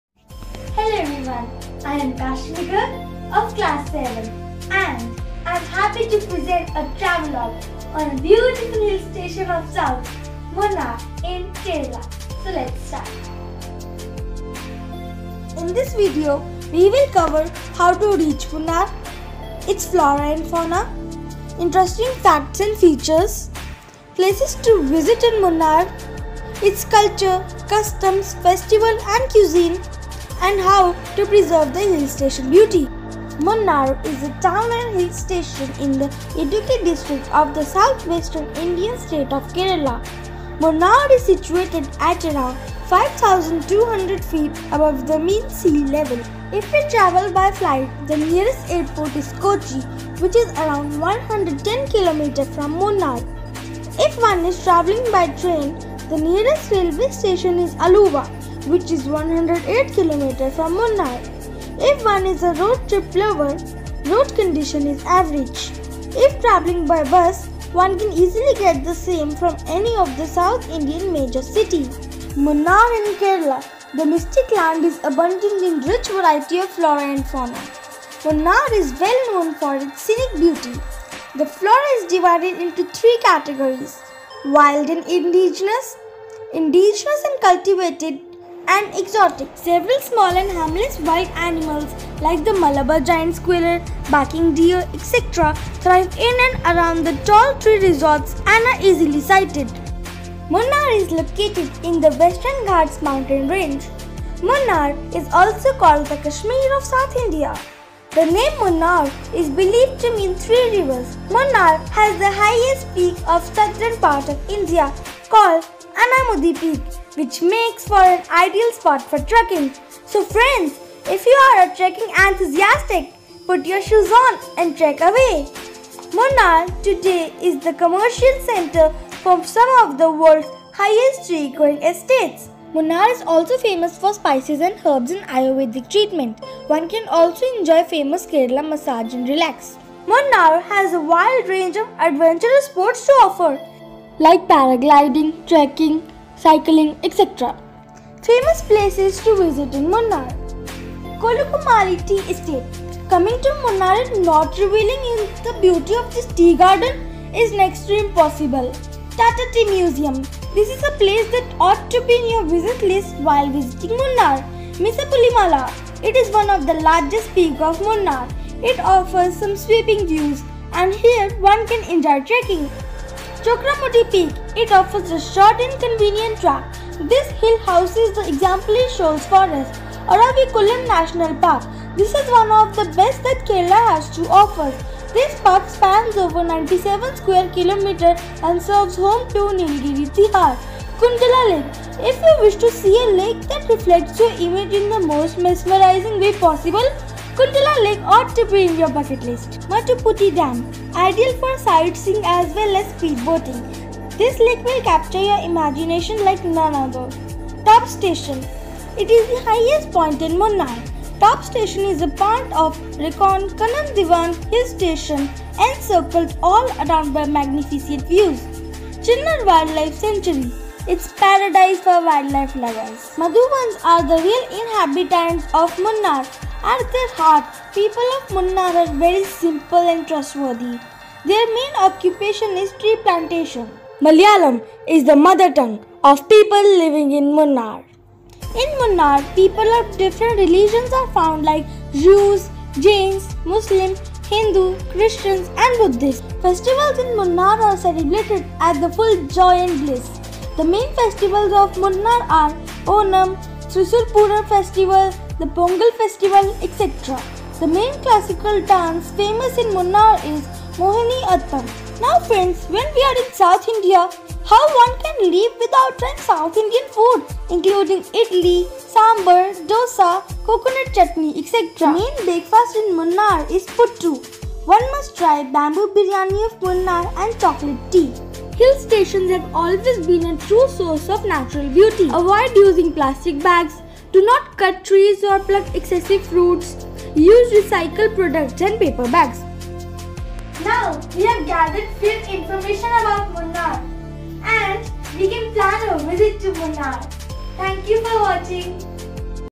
Munnar, Kerela Travel VLog
Description

Munnar Tourist Places | Munnar Tour Budget | Munnar Tour Complete Information | Munnar Kerala Trip

Munnar is situated in Kerala and this place is very popular among tourist because of its tea garden and beautiful mountains. South India is always attract tourists and specially kerala. In this series I covered some parts of kerala and I started from famous hill station munnar. This video will give you complete picture of munnar and all places with complete budget and with all tips. This video is divided into two parts so watch both parts for complete information. Munnar south india ka ek prasiddh hill station hai jaha desh bhar se tourist aate hai aur agar aap bhi apni chuttiya south india me plan kar rahe hai to ye video aapke lie kafi helpful hogi kyoki jyadatar tourist kerala ki apni trip me munnar ko jrur add karte hai aur ye video aapko munnar ke baare me sabhi jruri information degi jis se aap asani se munnar ki trip plan kar skte hai.